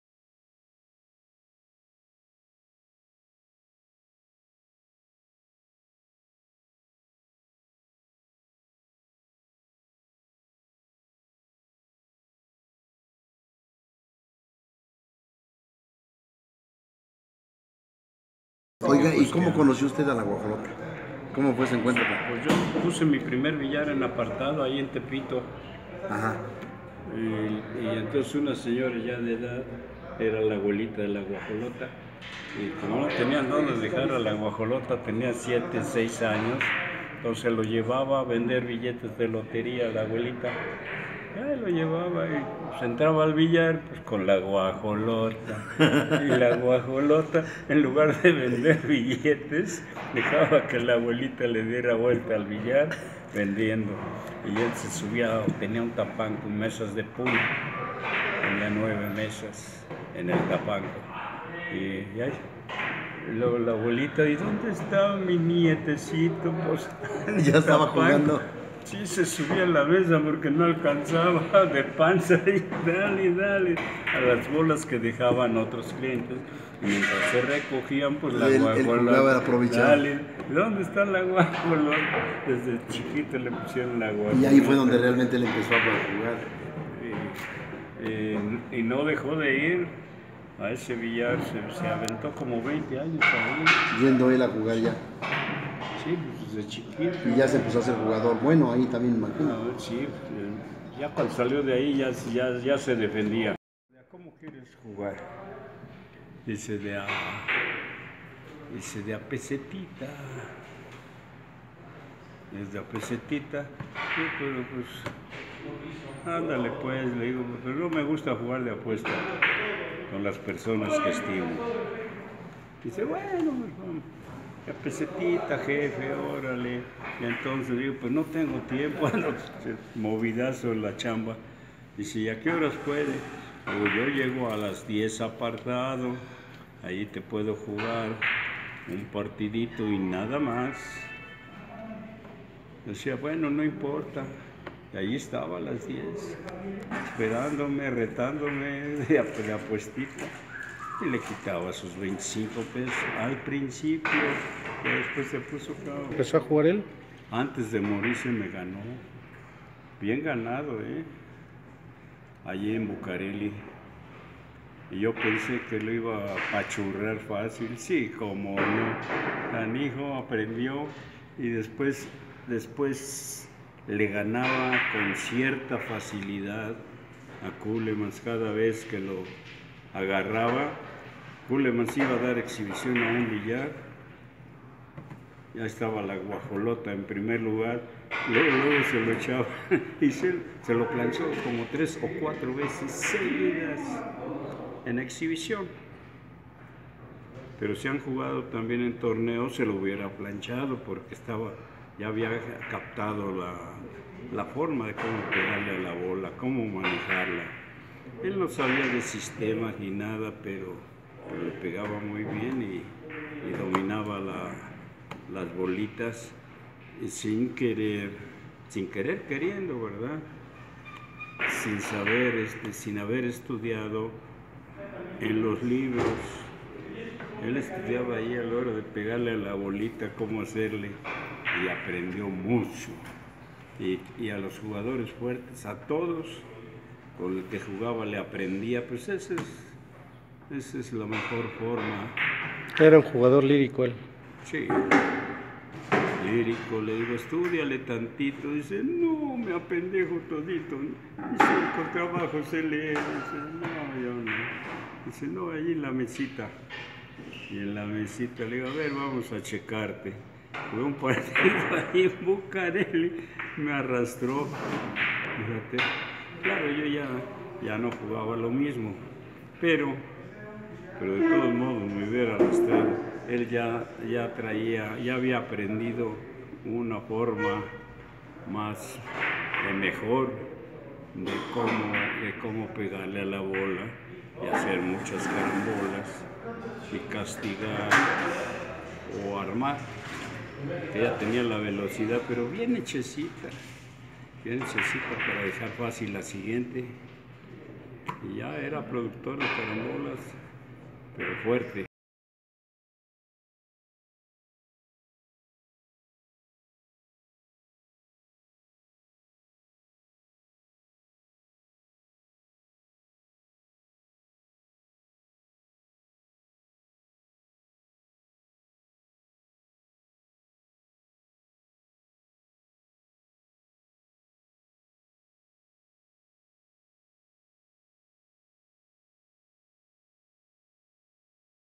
Las Glorias del billar Mexicano "Gabriel Fernández" PARTE 1
Las Glorias del billar Mexicano
"Gabriel Fernández"
Parte 1
Aurelio Pérez “La Guajolota” lo conocí desde los 6 o 7 años en mi primer billar que estaba ubicado en la calle de Apartado  en el barrio bravo de Tepito…..
Le decía estúdiale tantito….y me respondía NO!  Me apendejo todito…
Era un jugador de mucho talento..